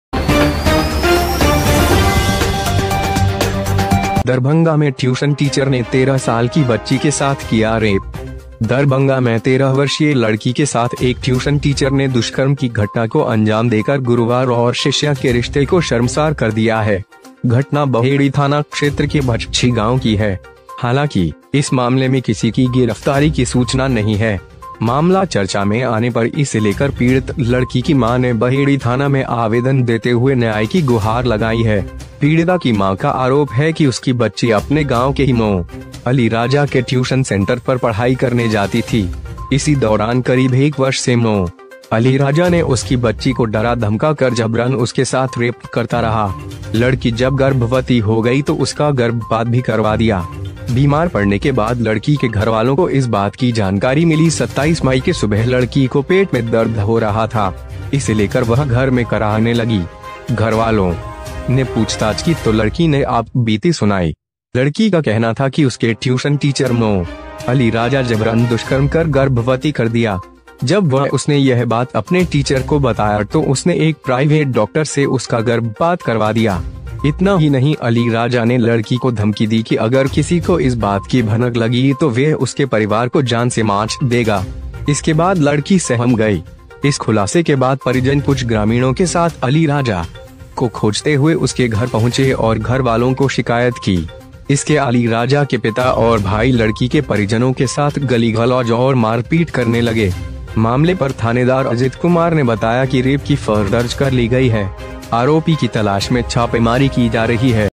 दरभंगा में ट्यूशन टीचर ने 13 साल की बच्ची के साथ किया रेप| Darbhanga crime news
दरभंगा में  ट्यूशन टीचर ने 13 साल की बच्ची के साथ किया रेप:

दरभंगा में 13 वर्षीय लड़की के साथ एक ट्यूशन टीचर ने दुष्कर्म की घटना को अंजाम देकर गुरु और शिष्या के रिश्ते को शर्मसार कर दिया है। घटना बहेड़ी थाना क्षेत्र के भचछि गांव की है। हालांकि, इस मामले में किसी की गिरफ्तारी की सूचना नहीं है। मामला चर्चा में आने पर इसे लेकर पीड़ित लड़की की मां ने बहेड़ी थाना में आवेदन देते हुए न्याय की गुहार लगाई है।
पीड़िता की मां का आरोप है कि उसकी बच्ची अपने गांव के ही मो. अली राजा के ट्यूशन सेंटर पर पढ़ाई करने जाती थी। इसी दौरान करीब एक वर्ष से मो. अलीराजा ने उसकी बच्ची को डरा-धमका कर जबरन उसके साथ रेप करता रहा। लड़की जब गर्भवती हो गई तो उसका गर्भपात भी करवा दिया। बीमार पड़ने के बाद लड़की के घरवालों को इस बात की जानकारी मिली।
27 मई की सुबह लड़की को पेट में दर्द हो रहा था। इसे लेकर वह घर में कराहने लगी। घरवालों ने पूछताछ की तो लड़की ने आपबीती सुनाई। लड़की का कहना था कि उसके ट्यूशन टीचर मो. अली राजा जबरन दुष्कर्म कर गर्भवती कर दिया। जब वह उसने यह बात अपने टीचर को बताया तो उसने एक प्राइवेट डॉक्टर से उसका गर्भपात करवा दिया। इतना ही नहीं अली राजा ने लड़की को धमकी दी कि अगर किसी को इस बात की भनक लगी तो वह उसके परिवार को जान से मार देगा। इसके बाद लड़की सहम गई।
इस खुलासे के बाद परिजन कुछ ग्रामीणों के साथ अली राजा को खोजते हुए उसके घर पहुंचे और घर वालों को शिकायत की। इसके अली राजा के पिता और भाई लड़की के परिजनों के साथ गली-गलौज और मारपीट करने लगे। मामले पर थानेदार अजित कुमार ने बताया कि रेप की FIR दर्ज कर ली गई है। आरोपी की तलाश में छापेमारी की जा रही है।

 Latest NEWS On DARBHANGA, Mithila, Bihar, India, politics, nation, entertainment, sports, science, Notification, New Rules & Job.